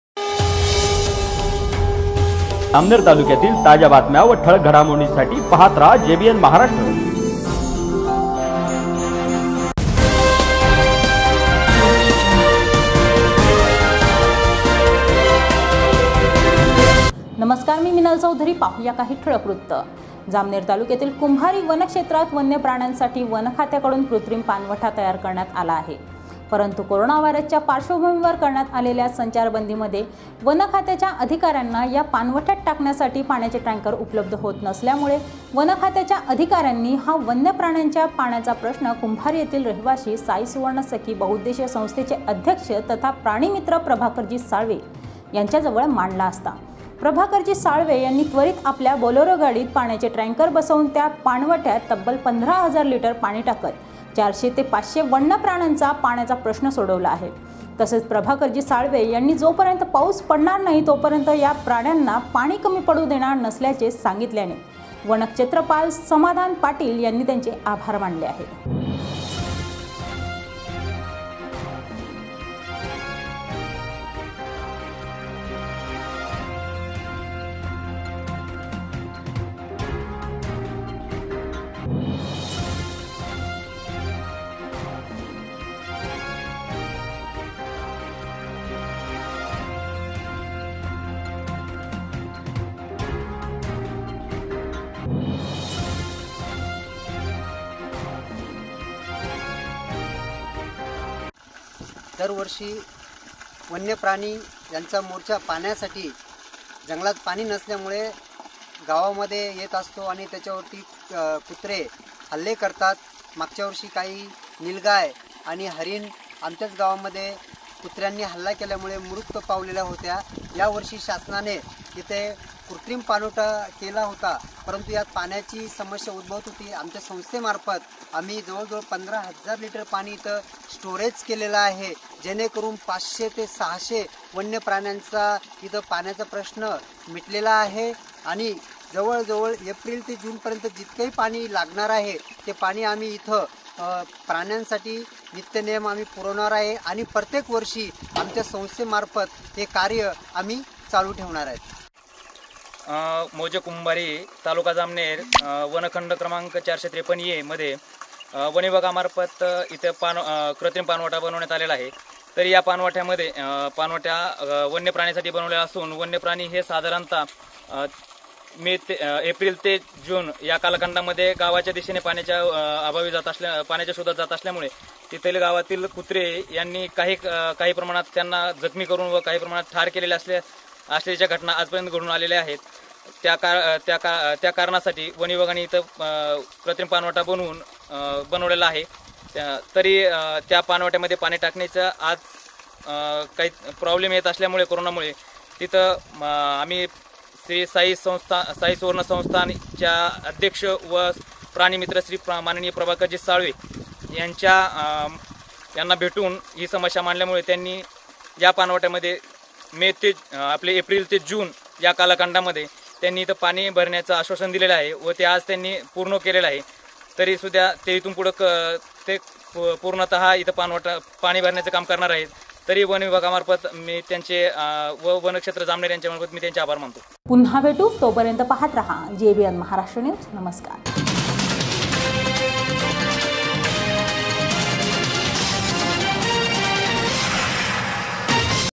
jbn maharashtra news jalgaon
वन्यप्राण्यांना पिण्याच्या पाण्याचा प्रश्न वन्य अधिकाऱ्यांची हाक ऐकत  प्रभाकर साळवे यांनी सोडवला वन्य प्राण्यांच्या पिण्याचे पाणी  पावसाळ्यापर्यंत पुरवण्याची घेतली हमी